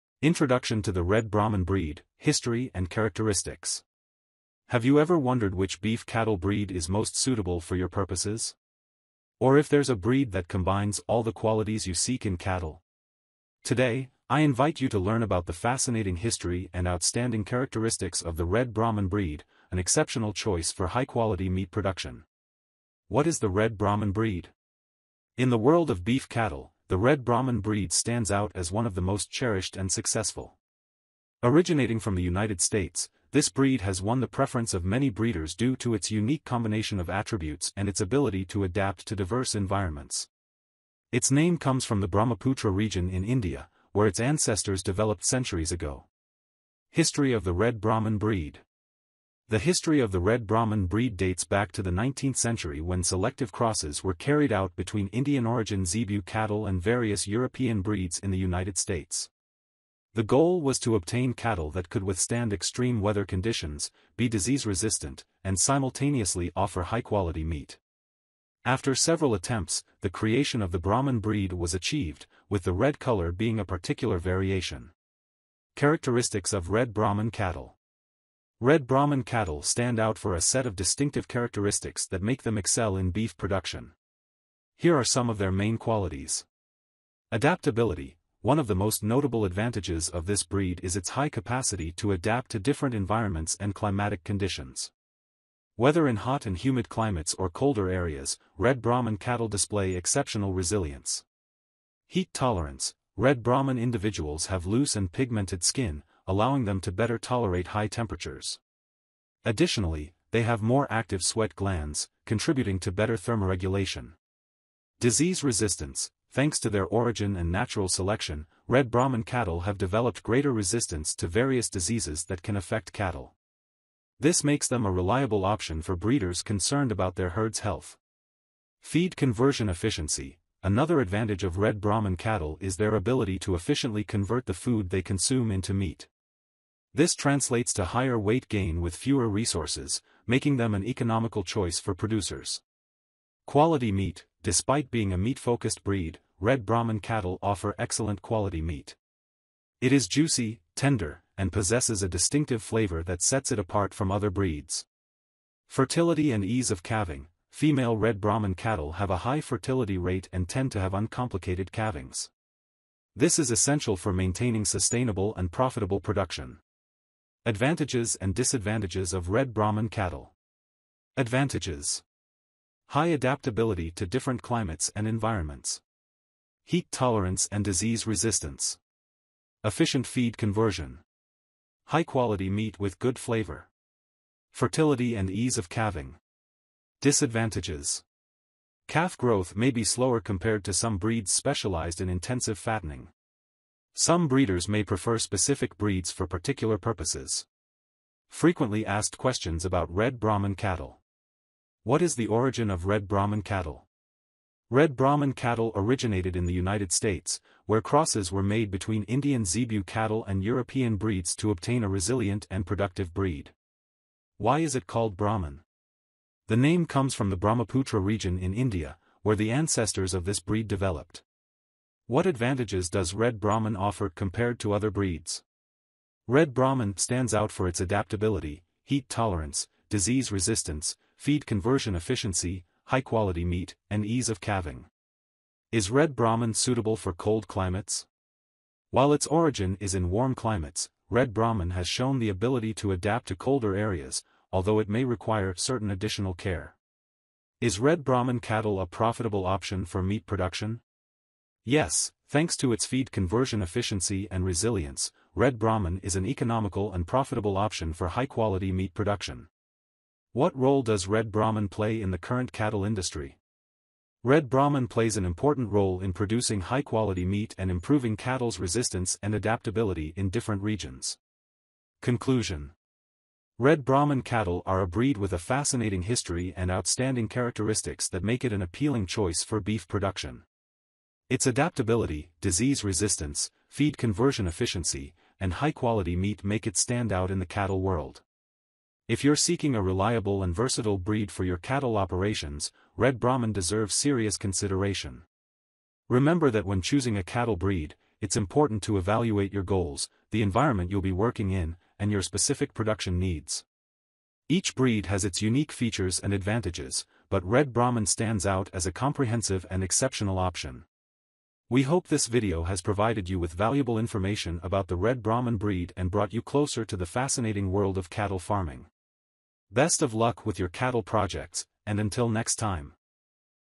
🔴 RED BRAHMAN Cattle Breed History And Characteristics
Have you ever wondered which beef cattle breed is most suitable for your purposes? Or if there's a breed that combines all the qualities you seek in cattle? Today, I invite you to learn about the fascinating history and outstanding characteristics of the Red Brahman breed, an exceptional choice for high-quality meat production.

What is the Red Brahman Breed?

In the world of beef cattle, the Red Brahman breed stands out as one of the most cherished and successful. Originating from the United States, this breed has won the preference of many breeders due to its unique combination of attributes and its ability to adapt to diverse environments. Its name comes from the Brahmaputra region in India, where its ancestors developed centuries ago.

History of the Red Brahman Breed

The history of the Red Brahman breed dates back to the 19th century when selective crosses were carried out between Indian-origin Zebu cattle and various European breeds in the United States. The goal was to obtain cattle that could withstand extreme weather conditions, be disease-resistant, and simultaneously offer high-quality meat. After several attempts, the creation of the Brahman breed was achieved, with the red color being a particular variation.

Characteristics of Red Brahman Cattle

Red Brahman cattle stand out for a set of distinctive characteristics that make them excel in beef production. Here are some of their main qualities:

Adaptability: One of the most notable advantages of this breed is its high capacity to adapt to different environments and climatic conditions. Whether in hot and humid climates or colder areas, Red Brahman cattle display exceptional resilience.

Heat Tolerance: Red Brahman individuals have loose and pigmented skin, allowing them to better tolerate high temperatures. Additionally, they have more active sweat glands, contributing to better thermoregulation.

Disease Resistance: Thanks to their origin and natural selection, Red Brahman cattle have developed greater resistance to various diseases that can affect cattle. This makes them a reliable option for breeders concerned about their herd's health.

Feed Conversion Efficiency: Another advantage of Red Brahman cattle is their ability to efficiently convert the food they consume into meat. This translates to higher weight gain with fewer resources, making them an economical choice for producers.

Quality Meat: Despite being a meat-focused breed, Red Brahman cattle offer excellent quality meat. It is juicy, tender, and possesses a distinctive flavor that sets it apart from other breeds.

Fertility and Ease of Calving: Female Red Brahman cattle have a high fertility rate and tend to have uncomplicated calvings. This is essential for maintaining sustainable and profitable production.

Advantages and Disadvantages of Red Brahman Cattle

Advantages:

High adaptability to different climates and environments.
Heat tolerance and disease resistance.
Efficient feed conversion.
High-quality meat with good flavor.
Fertility and ease of calving.
Disadvantages:

Calf growth may be slower compared to some breeds specialized in intensive fattening.
Some breeders may prefer specific breeds for particular purposes.

Frequently Asked Questions about Red Brahman Cattle

What is the origin of Red Brahman cattle?

Red Brahman cattle originated in the United States, where crosses were made between Indian Zebu cattle and European breeds to obtain a resilient and productive breed.

Why is it called Brahman?

The name comes from the Brahmaputra region in India, where the ancestors of this breed developed.

What advantages does Red Brahman offer compared to other breeds?

Red Brahman stands out for its adaptability, heat tolerance, disease resistance, feed conversion efficiency, high-quality meat, and ease of calving.

Is Red Brahman suitable for cold climates?

While its origin is in warm climates, Red Brahman has shown the ability to adapt to colder areas, although it may require certain additional care.

Is Red Brahman cattle a profitable option for meat production?

Yes, thanks to its feed conversion efficiency and resilience, Red Brahman is an economical and profitable option for high-quality meat production.

What role does Red Brahman play in the current cattle industry?

Red Brahman plays an important role in producing high-quality meat and improving cattle's resistance and adaptability in different regions.